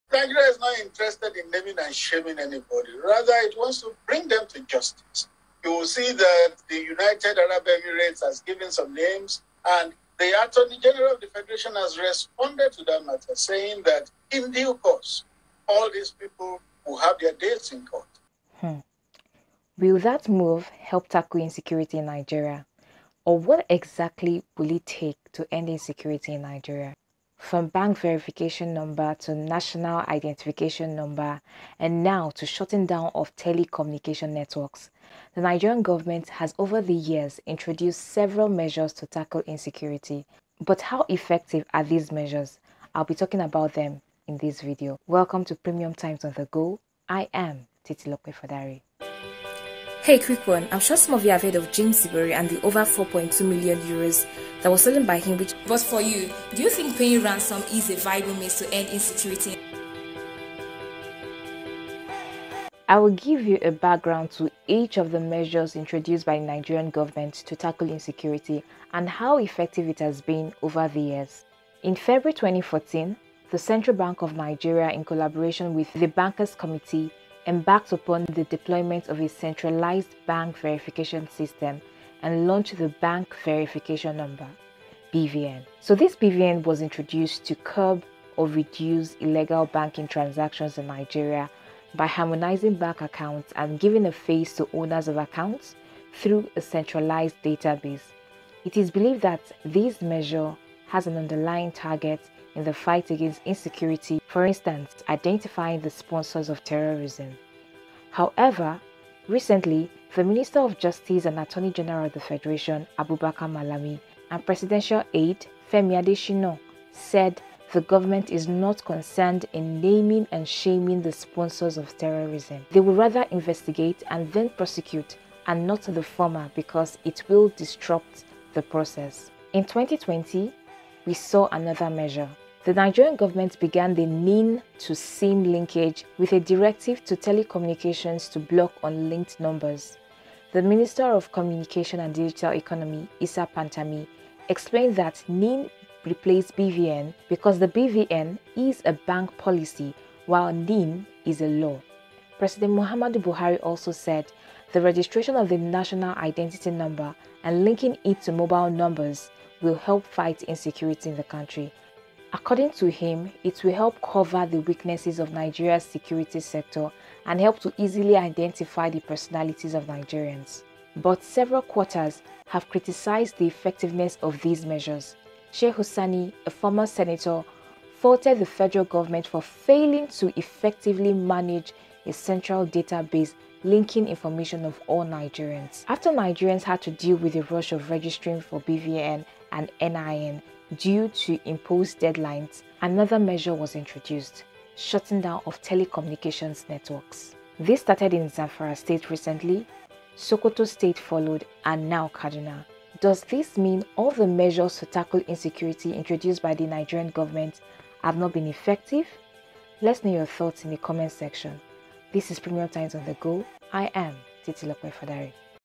PT On the Go: Non-combative measures in fighting insecurity
Over the years, the Nigerian Government has introduced several measures to tackle insecurity.

From Bank Verification Number to National Identification Number and now the shutting down of Telecommunications Networks in some states.